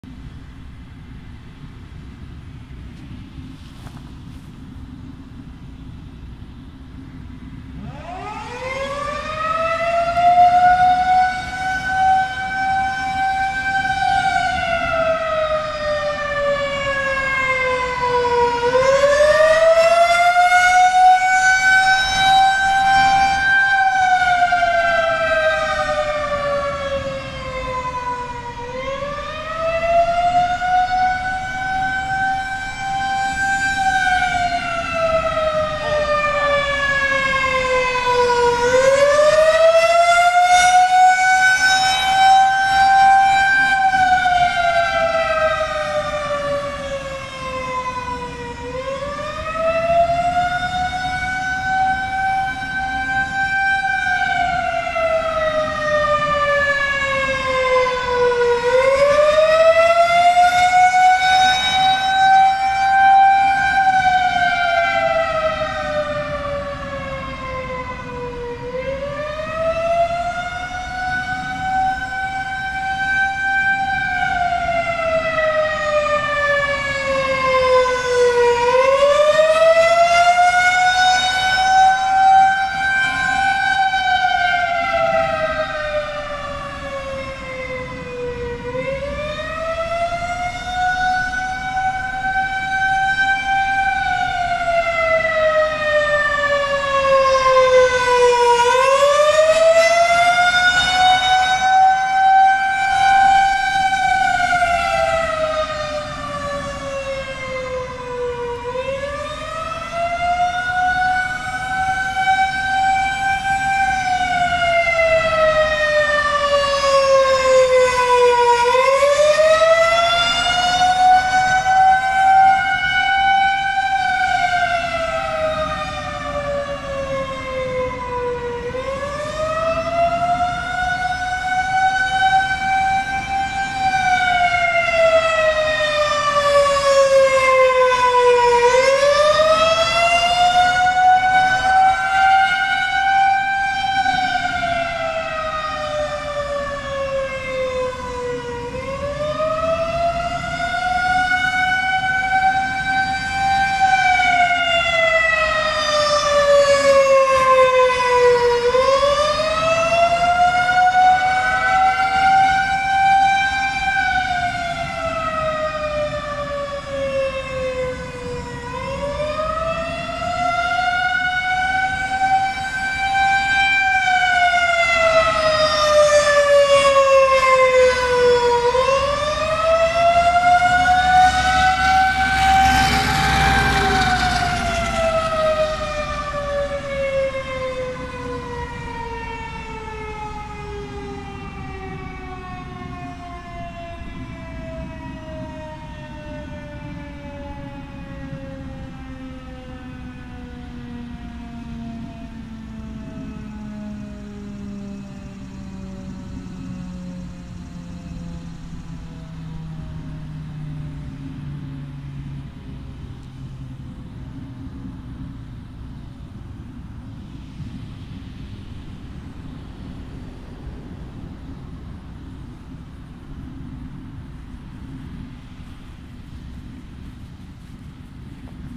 FS 2001-SRNB Siren Test | Full Attack | Batesville, IN
**Recorded 8/5/23**

So we weren't originally planning on heading to Batesville. We were gonna originally head to Clinton County (specifically Blanchester), however we realized that we weren’t gonna make it there in time, so Batesville was our best bet. Even then, we got to Batesville about 6 minutes before they tested. Thankfully, that gave me some time to set up my cameras. I also wanted to try something different with this video and have my tight shot purposely off-centered. I usually don't do that, but in this case it doesn't look all that bad. As a bonus, I also decided to add in a little undershot clip towards the end of the video. I don't plan on doing shots like this regularly, but I just wanted to switch things up a little bit with this one.

*Additional Info*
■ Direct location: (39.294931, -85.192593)
■ Testing: Every Saturday @ noon
■ Recording distance: 218 ft
■ Main shot: Panasonic Lumix DC-FZ80
■ Second shot: Sony CyberShot DSC-HX20V
■ Audio: Zoom H1N Portable Audio Recorder
■ Enthusiasts present: Aiden (@SportyTrainGuy), Dominic (@SuperSirex127), Benicio (@CentralOHSirens)